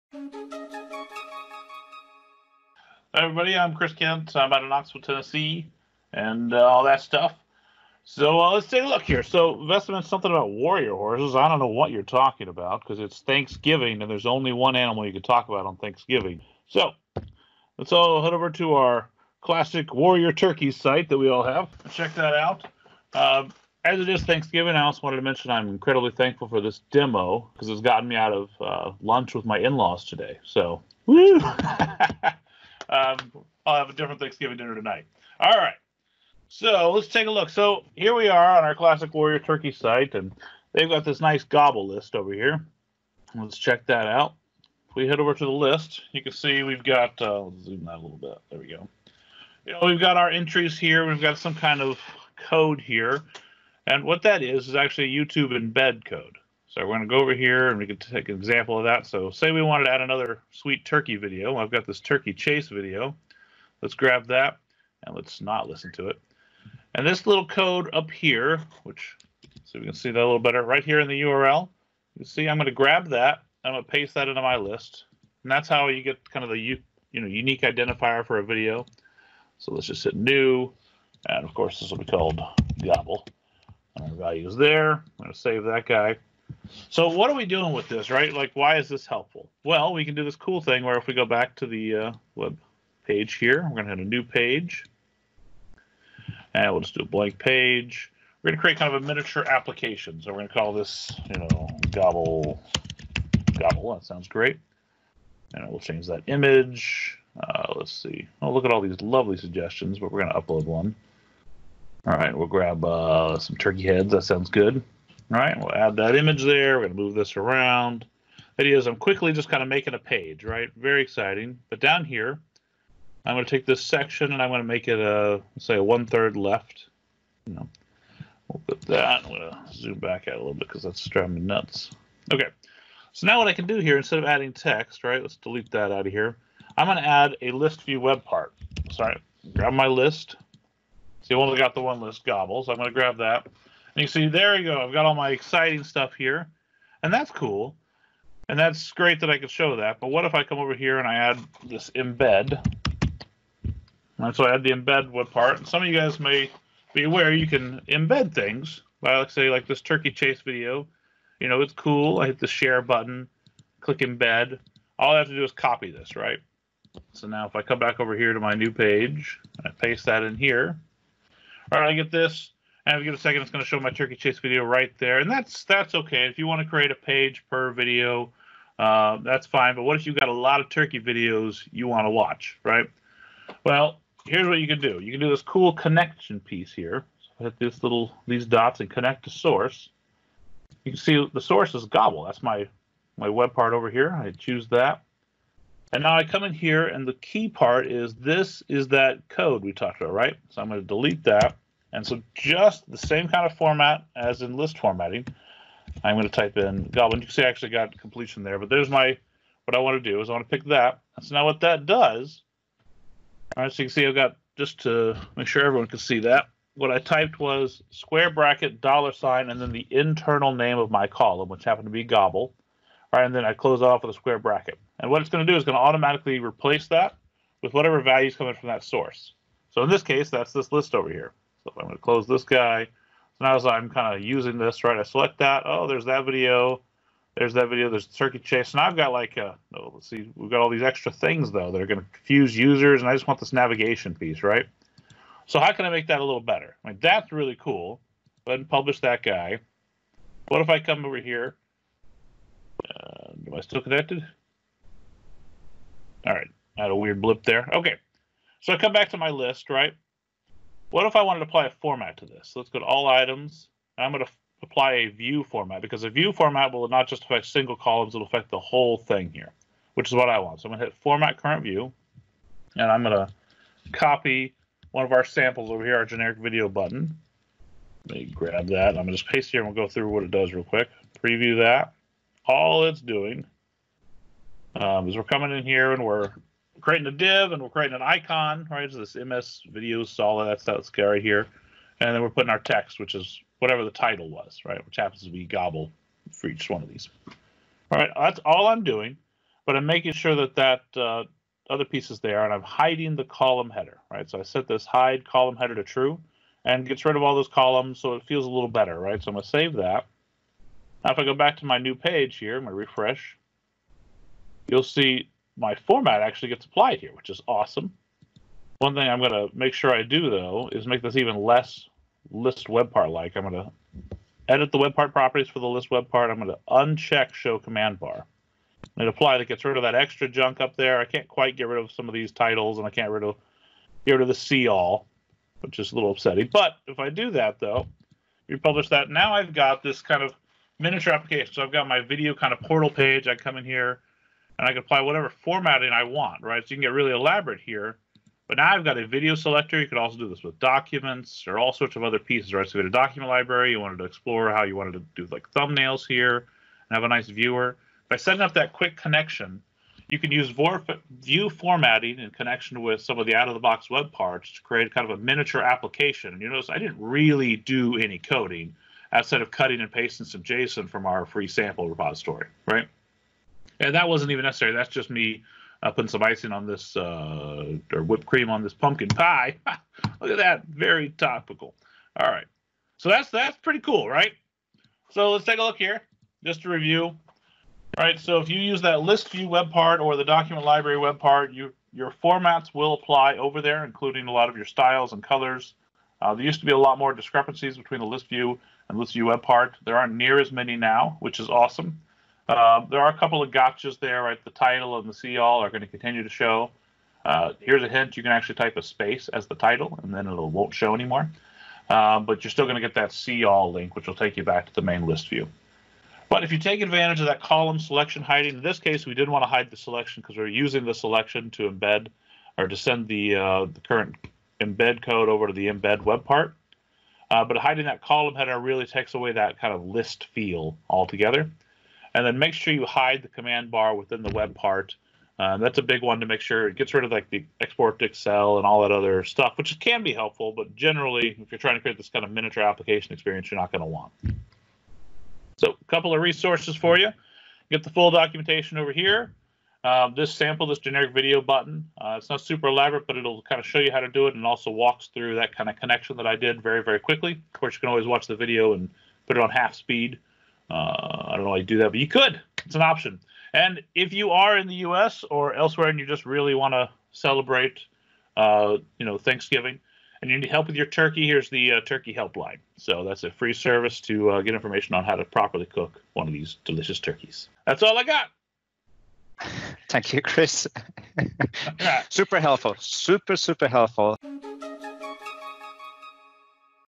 Updated capabilities on SharePoint column and view formatting
This PnP community demo is taken from the bi-weekly SharePoint Developer Community - General Development Special Interest Group (SIG) call recorded on November 28, 2019.

In this 8-minute video, Chris Kent show how to configure the List view web part and Embed web part to work together like an application that allows the user to select and display documents, photos, videos, etc., in the right pane of a web page.

Presenter:  Chris Kent (DMI) | @theChrisKent